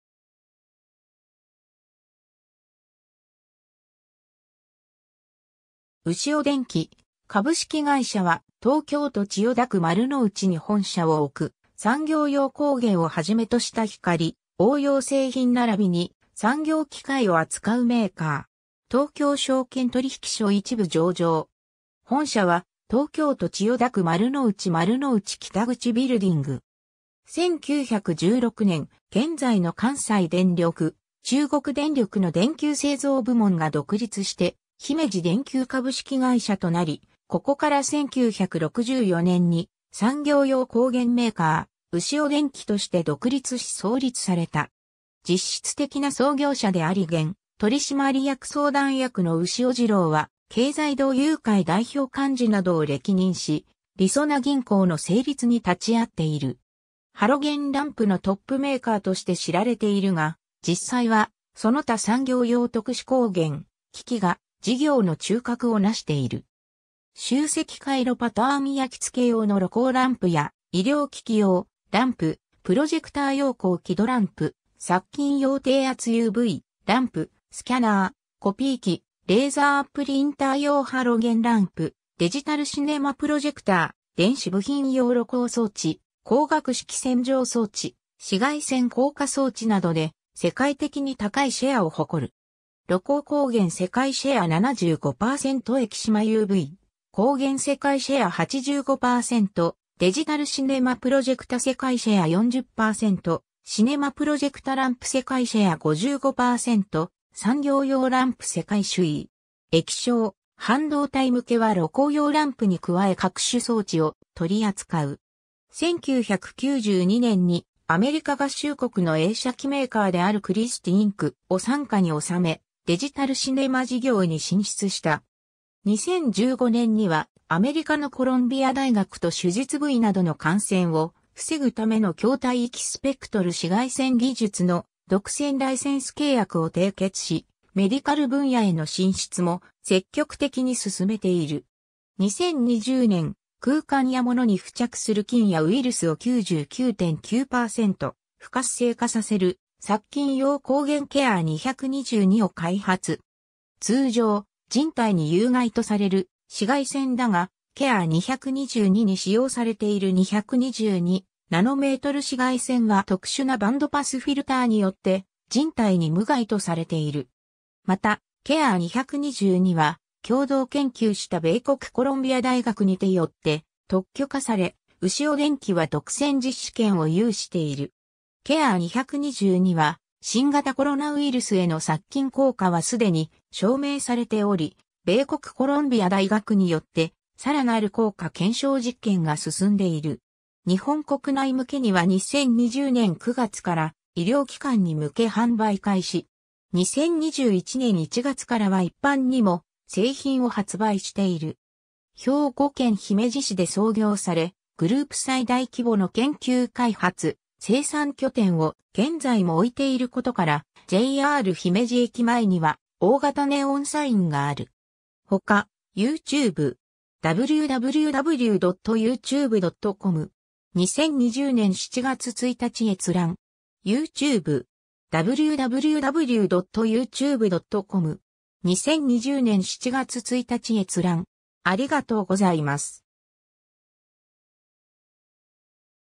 ウシオ電機
ウシオ電機株式会社（ウシオでんき、英文名称: Ushio Inc.）は、東京都千代田区丸の内に本社を置く、産業用光源をはじめとした光応用製品ならびに産業機械を扱うメーカー。
東京証券取引所一部上場。
 本社は、東京都千代田区丸の内 丸の内北口ビルディング。
 1916年、現在の関西電力・中国電力の電球製造部門が独立して姫路電球株式会社となり（後のウシオ工業株式会社）、ここから1964年に産業用光源メーカー、ウシオ電機として独立し創立された。
 実質的な創業者であり現取締役相談役の牛尾治朗は経済同友会代表幹事などを歴任し、りそな銀行の成立に立ち会っている。
 ハロゲンランプのトップメーカーとして 知られているが、実際はその他産業用特殊光源・機器が事業の中核をなしている。
集積回路パターン焼付け用の露光ランプや、医療機器用ランプ、プロジェクター用高輝度ランプ、殺菌用低圧UVランプ、スキャナー・コピー機・レーザープリンター用ハロゲンランプ、デジタルシネマプロジェクター、電子部品用露光装置、光学式洗浄装置、紫外線硬化装置などで 世界的に高いシェアを誇る。
 露光光源 世界シェア 75% エキシマUV光源 世界シェア 85% デジタルシネマプロジェクタ 世界シェア 40% シネマプロジェクタランプ 世界シェア 55% 産業用ランプ世界首位。
 液晶・半導体向けは露光用ランプに加え各種装置を取り扱う。
 1992年（平成4年）にアメリカ合衆国の映写機メーカーである CHRISTIE INC. (現: CHRISTIE DIGITAL SYSTEMS) を傘下に収め、デジタルシネマ事業に進出した。
 2015年にはアメリカのコロンビア大学と手術部位などの感染を防ぐための狭帯域スペクトル紫外線技術の独占ライセンス契約を締結し、メディカル分野への進出も積極的に進めている。
 2020年、空間や物に付着する菌やウイルスを (新型コロナウイルス、O-157、インフルエンザなど含む) 99.9%不活性化させる殺菌用光源 Care222を開発。
 通常、人体に有害とされる紫外線だが、Care222 に使用されている 222 nm 紫外線は特殊なバンドパスフィルターによって人体に無害とされている。
 またCare222は共同研究した米国コロンビア大学にてよって特許化され、ウシオ電機は独占実施権を有している。
 Care222は、新型コロナウイルス（Covid-19）への殺菌効果はすでに証明されており、米国コロンビア大学によって更なる効果検証実験が進んでいる。
 日本国内向けには2020年9月から医療機関に向け販売開始。
2021年１月からは一般にも製品を発売している。
 兵庫県姫路市で創業され、グループ最大規模の研究開発・生産拠点を現在も置いていることから、JR姫路駅前には大型ネオンサインがある。
ほか ^ “YouTube”. 2020年7月1日閲覧。